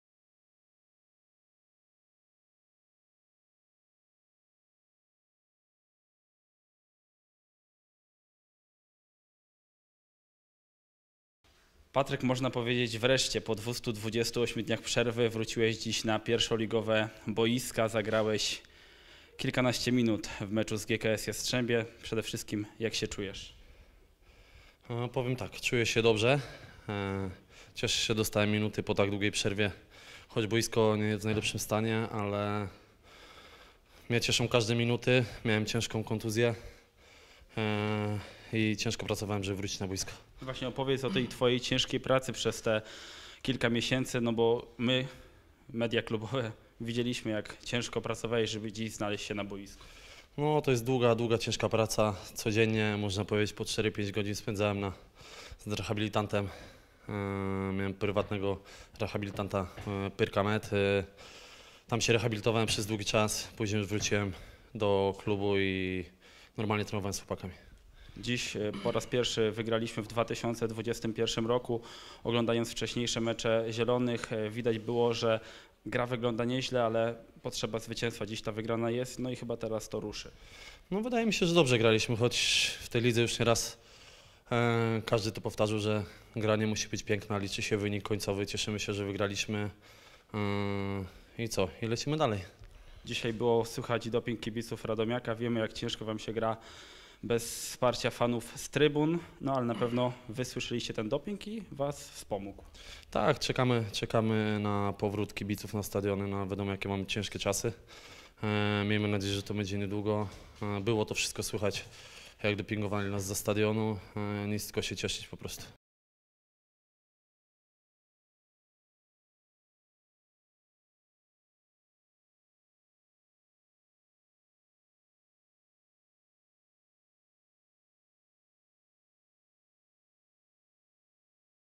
Patryk Mikita po meczu z GKS-em Jastrzębie [RADOMIAK.TV]
Rozmowa z piłkarzem Radomiaka - Patrykiem Mikitą po meczu z GKS Jastrzębie. Zieloni wygrali 2:0, a skrzydłowy Zielonych wrócił do gry po 228 dniach przerwy spowodowanej ciężką kontuzją.